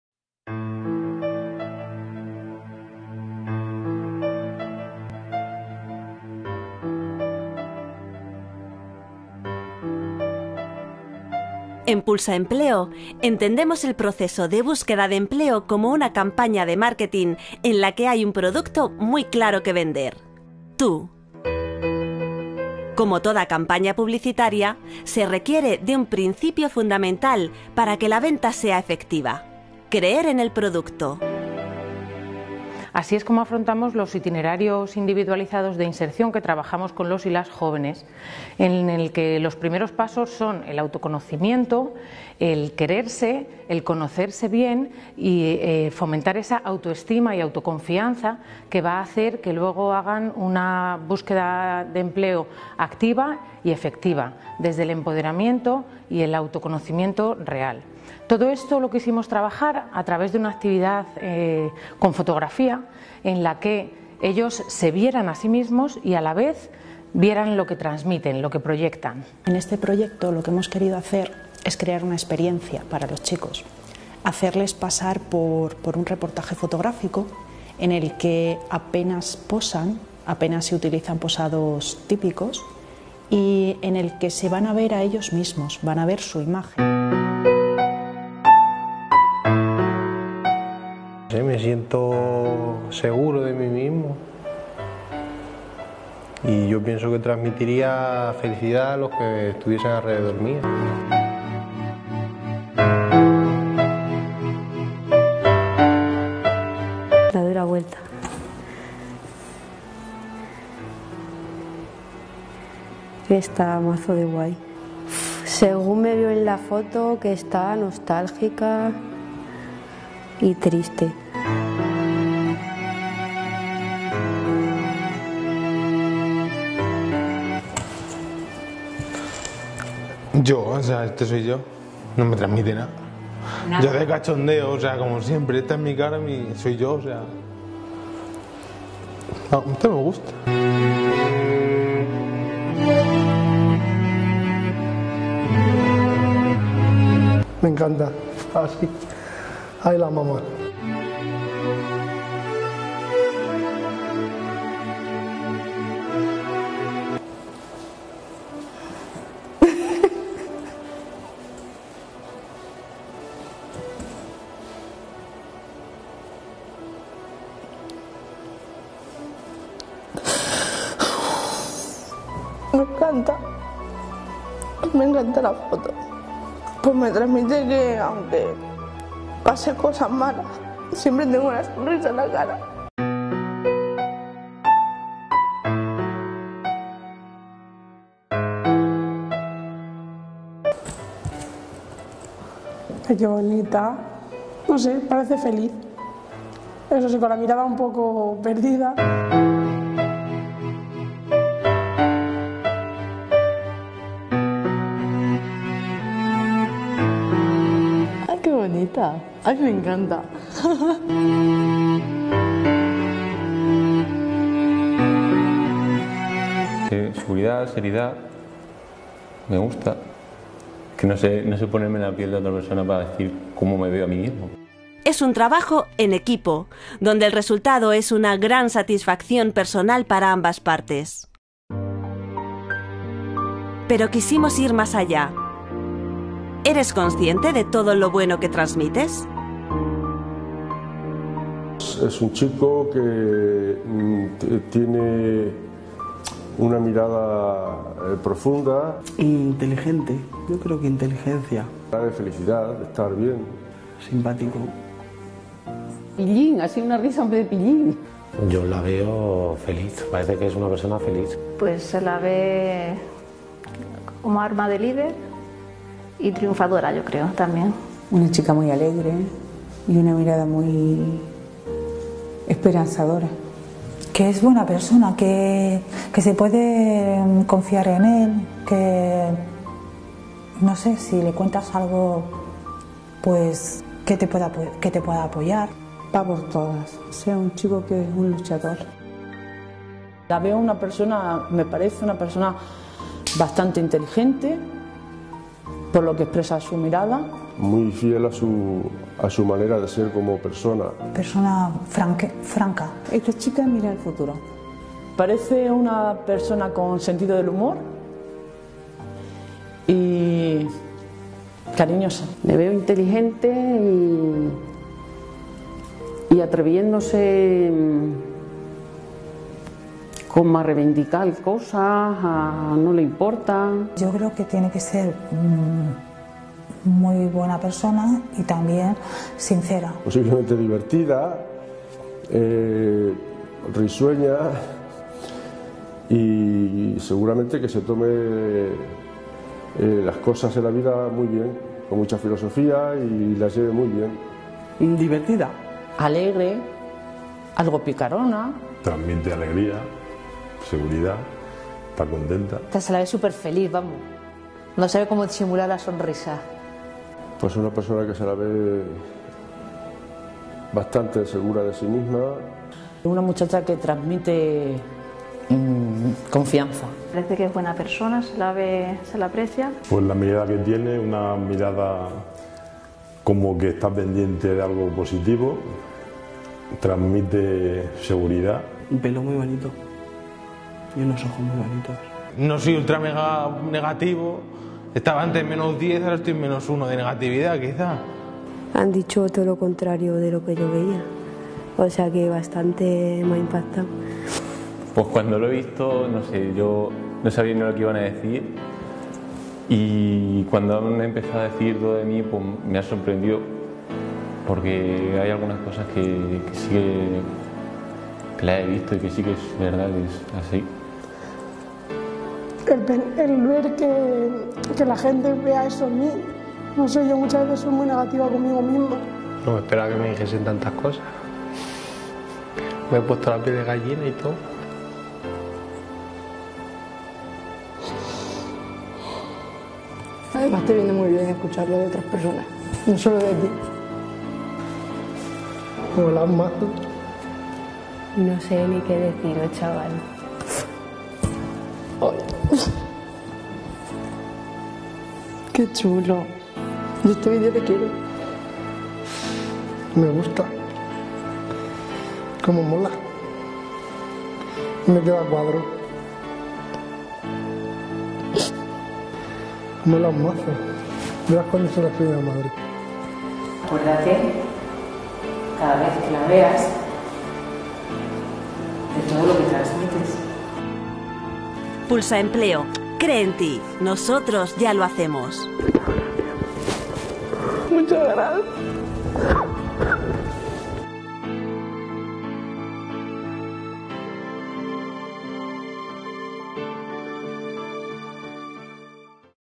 PulsaEmpleo: Jóvenes Sobradamente Competentes
Taller de Autoimagen, Fotografía y Empoderamiento para Jóvenes
PulsaEmpleo: Proyecto de Activación, orientación y acompañamiento sociolaboral para Jóvenes (Garantía Juvenil).

PulsaEmpleo es un proyecto de Cruz Roja en el Programa Operativo de Empleo Juvenil (POEJ), cofinanciado por el Fondo Social Europeo y el Ministerio de Empleo y Seguridad Social del Gobierno de España.